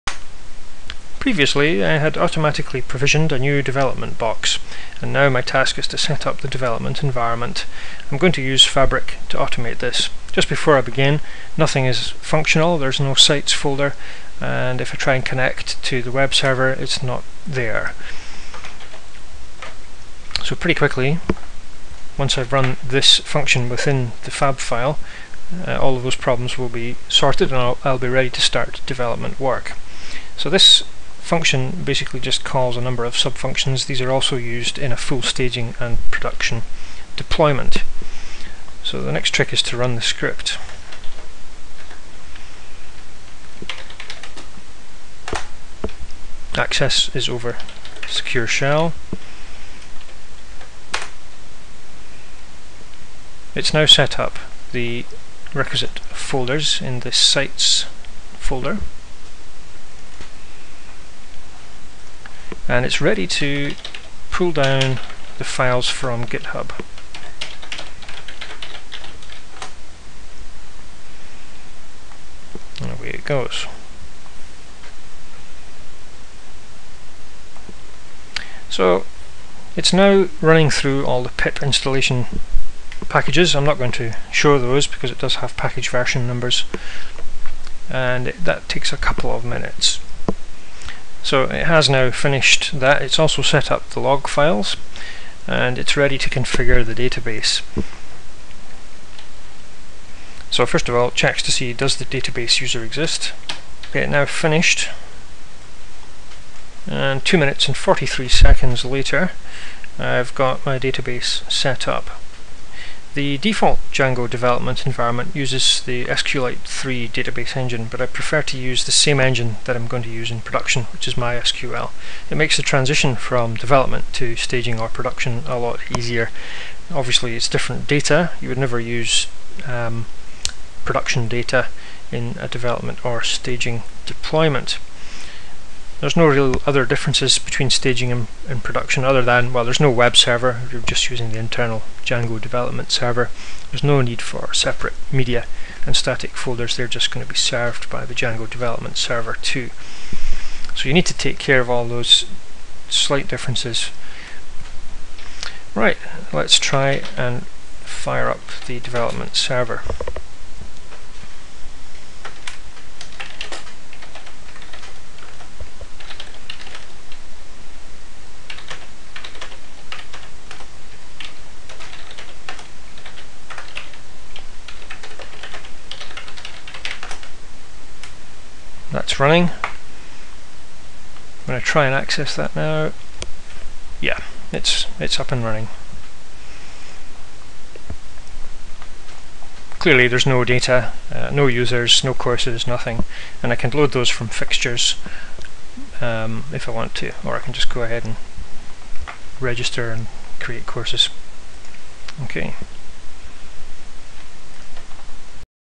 Dev. box deployment using Fabric
Using Fabric tools to automate the setup of a development environment.